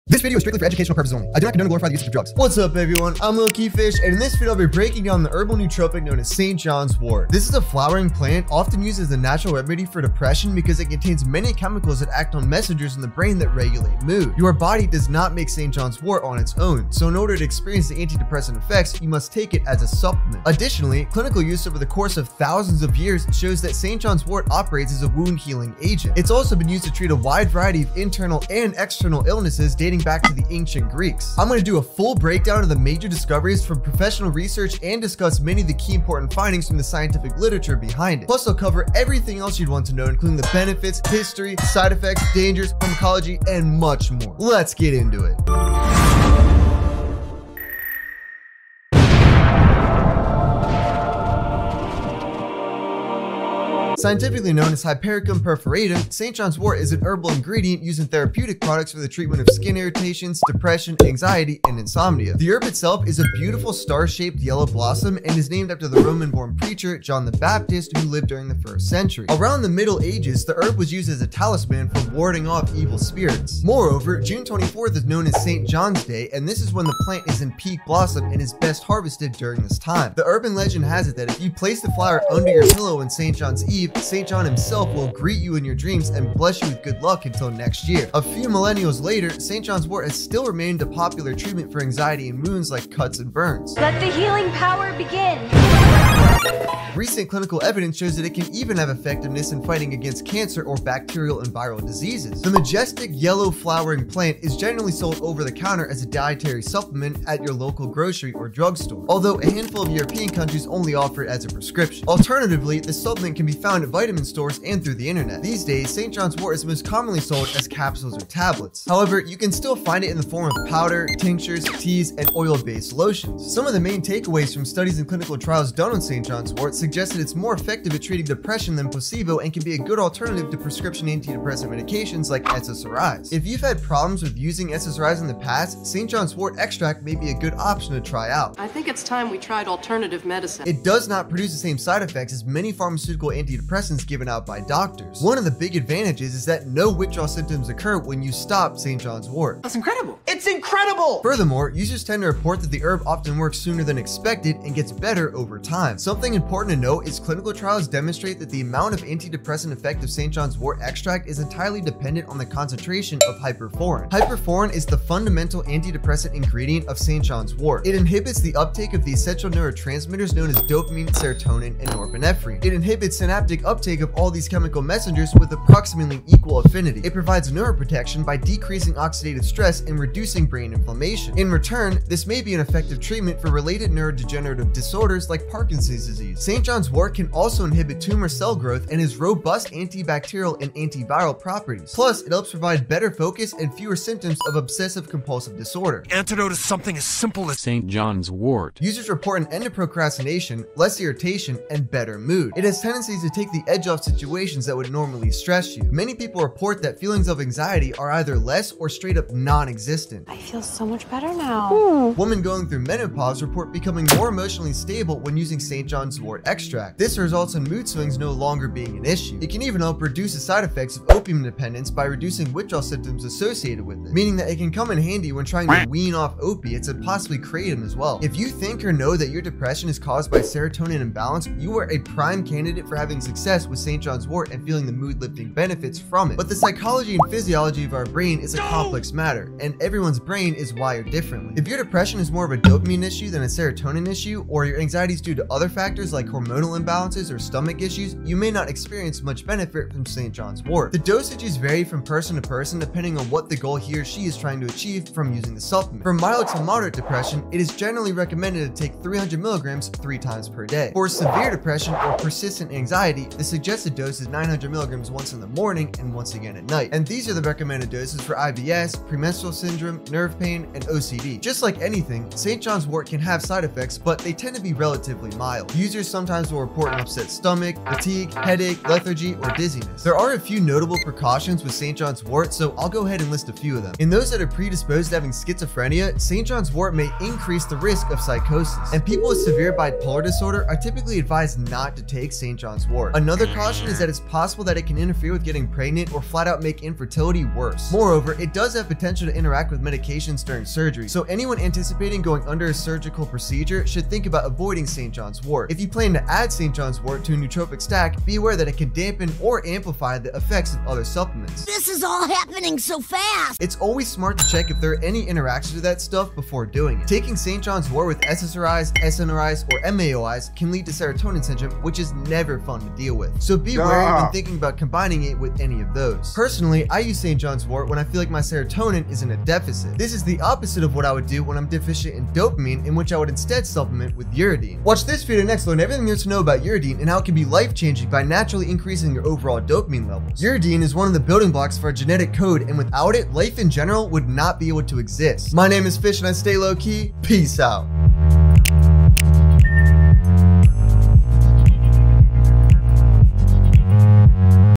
𝗦𝗧. 𝗝𝗢𝗛𝗡'𝗦 𝗪𝗢𝗥𝗧 Review - Flower With Healing Powers || Natural Remedy for Anxiety 🌼
I review St. John's Wort! St. John's Wort (Hypericum Perforatum) is a medicinal herb that has been used for centuries to treat various ailments such as depression, anxiety, and insomnia.

Chapters:
0:00 - What to expect
0:53 - History & Origin
1:42 - Where people get it
2:03 - Prescription Medications vs St. John's Wort
2:35 - How it works
2:56 - Benefits
3:48 - Will it work for you?
4:10 - Dosages
4:31 - Potential Side Effects
5:26 - Personal Review (My Experience)